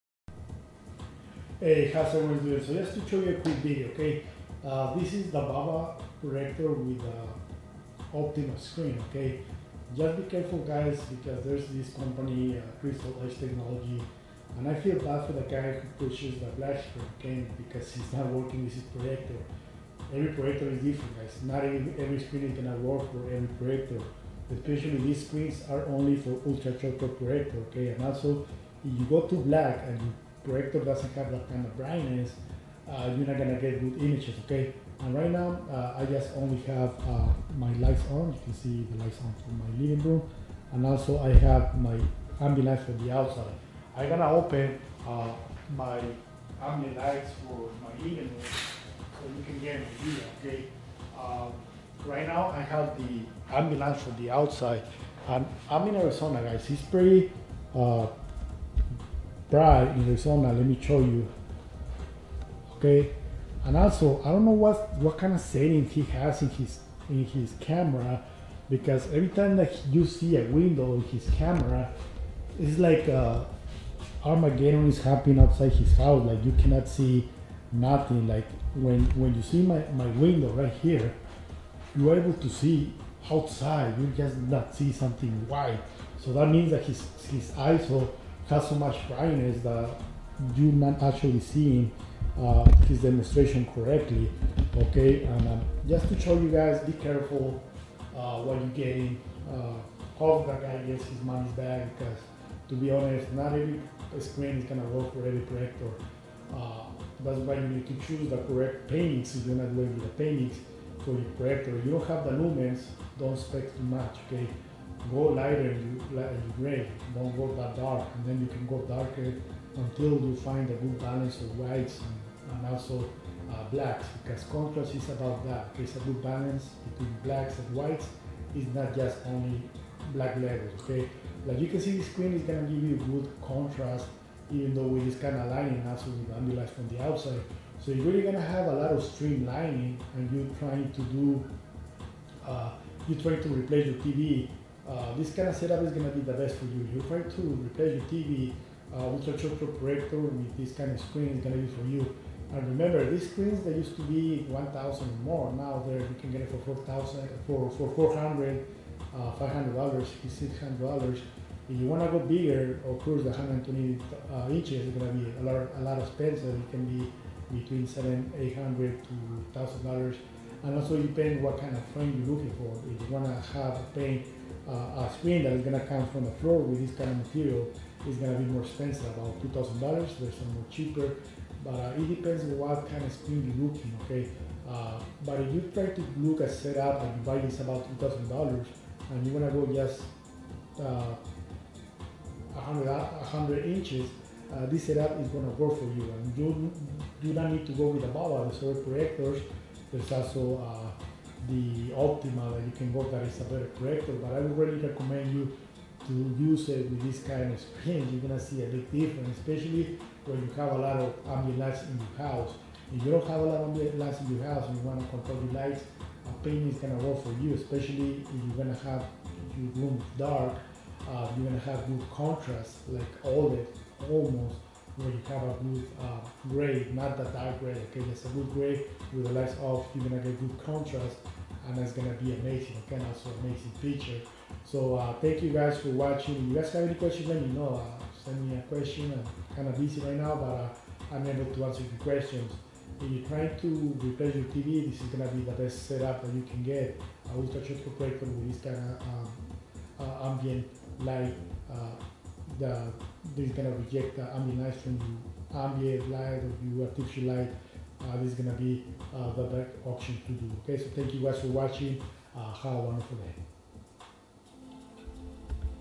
Projector screen is not for every projector. Stop the non sense
every projector is different. Difficult to make one screen to work with every projector. If screen does not work with super bright projector like the VAVA 4K then screen may not work for your projector.